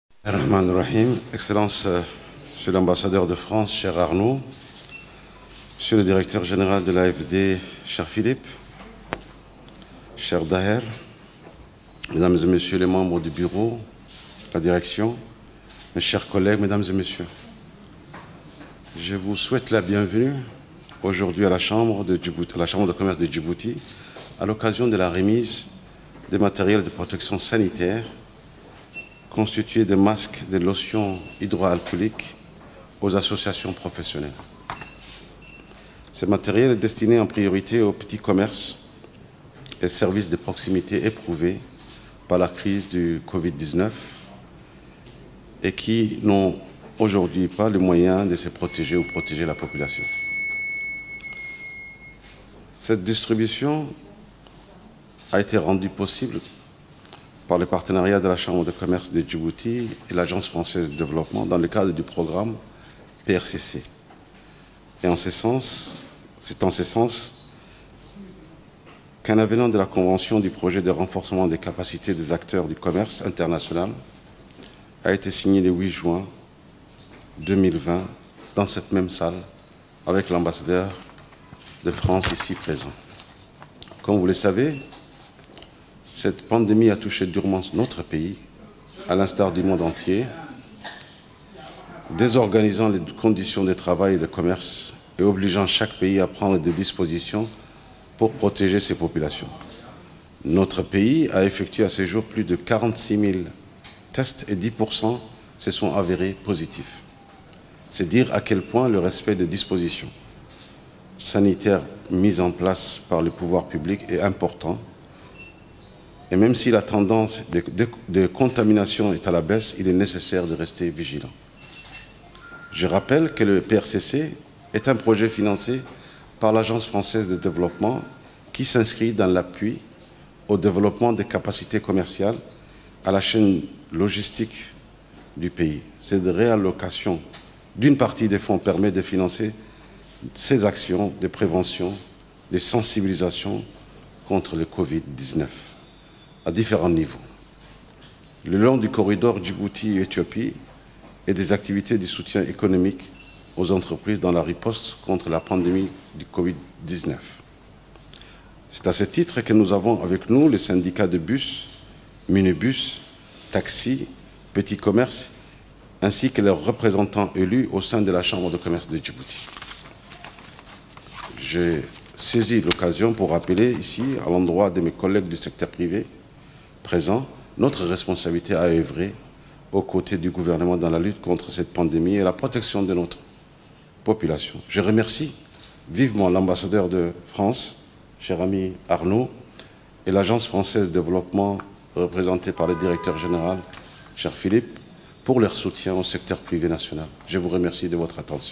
Ecoutons le discours du président de la Chambre de commerce de Djibouti, Youssouf Moussa Dawaleh
Ce jeudi 02 juillet 2020 à 10h, à la salle de conférence de la Chambre de Commerce de Djibouti, a eu lieu une cérémonie de remise de masques et de gels désinfectants.Le Président de la Chambre de Commerce de Djibouti, Monsieur Youssouf Moussa Dawaleh, l’Ambassadeur de France auprès de la République de Djibouti, Monsieur Arnaud Guillois et le Directeur de l’Agence Française de Développement à Djibouti, Monsieur Philippe Collignon, ont remis 2664 boites de masques soit 110.000 masques et 111 cartons de gels soit 2664 gels hydroalcooliques aux acteurs économiques intervenants le long du corridor commercial entre Djibouti et Addis Abeba, les postes frontières entre les deux pays ainsi que les entreprises du secteur du transport et de la logistique.